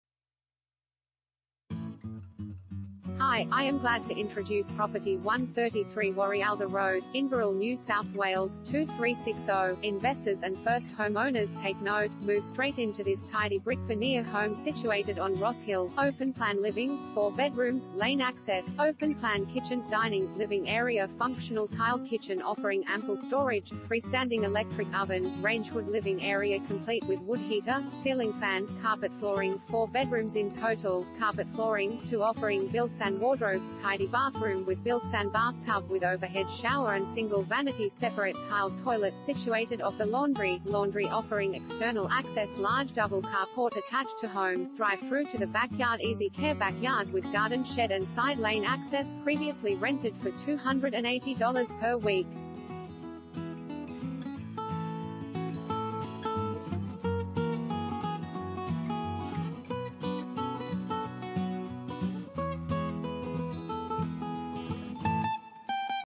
133 Warialda Road, Inverell NSW 2360 | AuPropertyReport.Com
133 Warialda Road, Inverell NSW 2360
Property Type: house
Bedrooms: 4
Bathrooms: 4
Car Space: 2
Investors &amp; First Home Owners Take Note